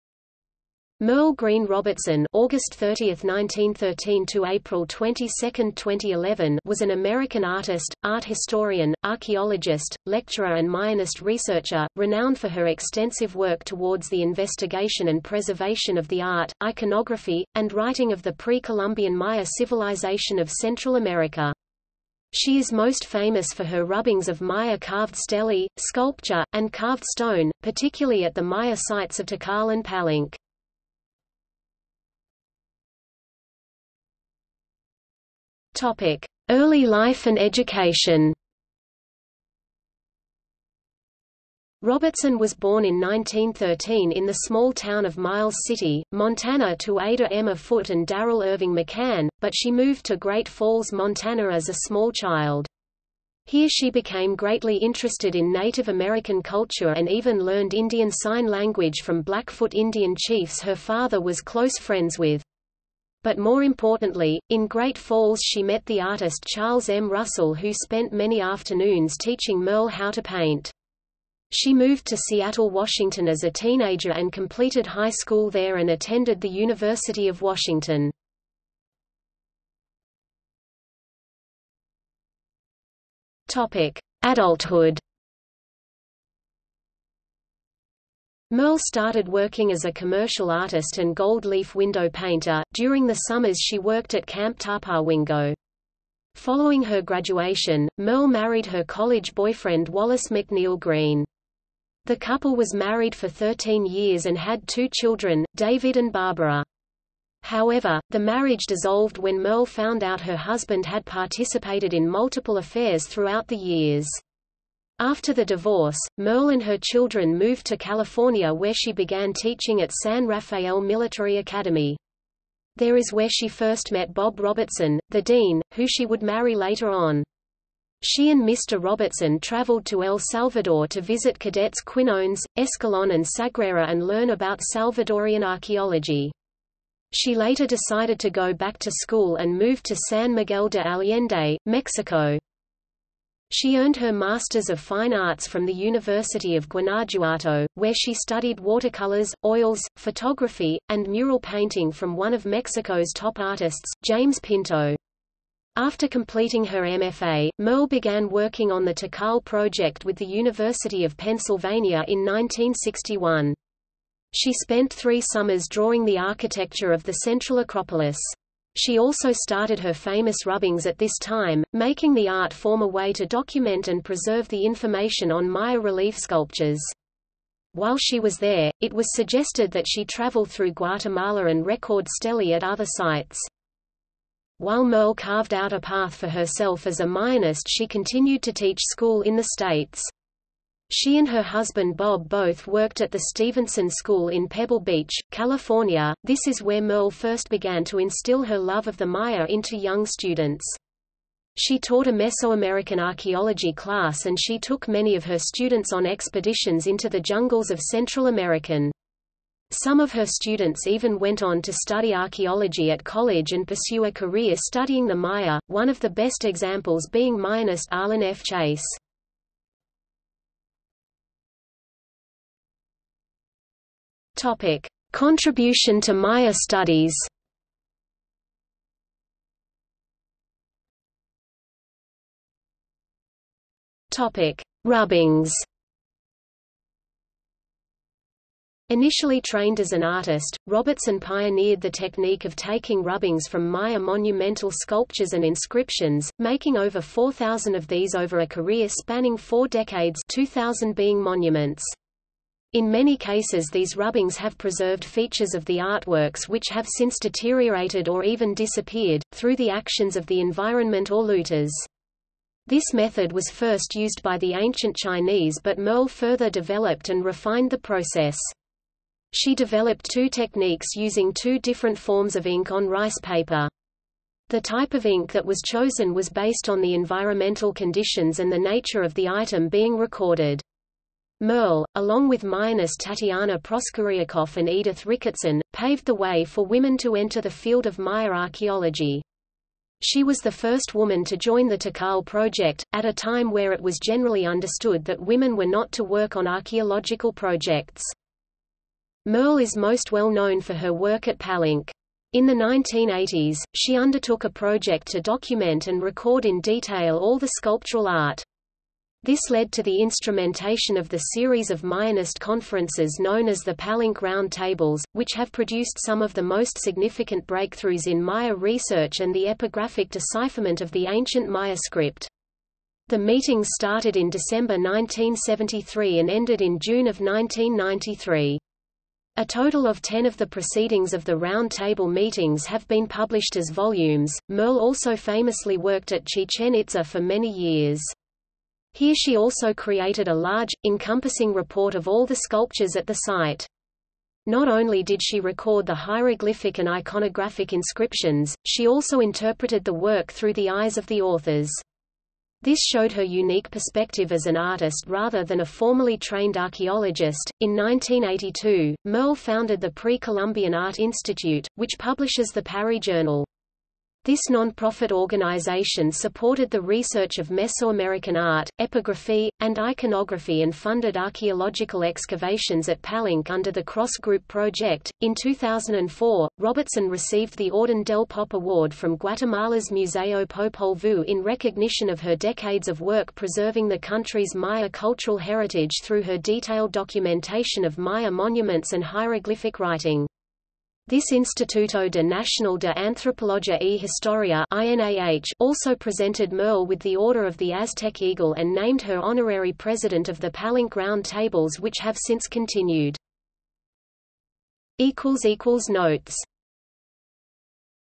Merle Greene Robertson | Wikipedia audio article
00:00:32 1 Early life and education
00:01:20 2 Adulthood
00:03:40 3 Contribution to Maya Studies
00:03:50 3.1 Rubbings

- Socrates

SUMMARY
Merle Greene Robertson (August 30, 1913 – April 22, 2011) was an American artist, art historian, archaeologist, lecturer and Mayanist researcher, renowned for her extensive work towards the investigation and preservation of the art, iconography, and writing of the pre-Columbian Maya civilization of Central America. She is most famous for her rubbings of Maya carved stelae, sculpture, and carved stone, particularly at the Maya sites of Tikal and Palenque.